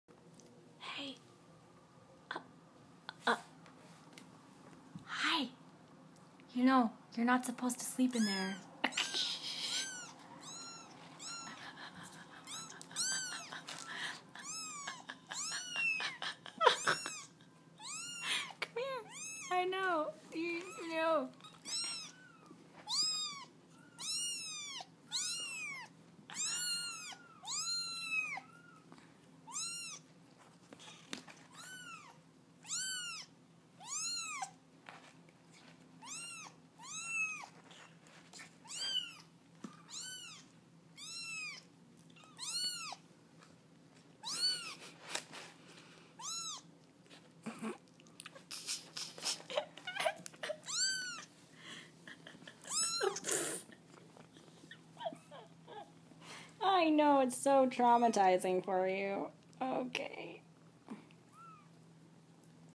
Litter Box Escape!
And trying to litter train them isn't going well either.  (we do have ramps! They're just not using them.) Aged about 4 weeks.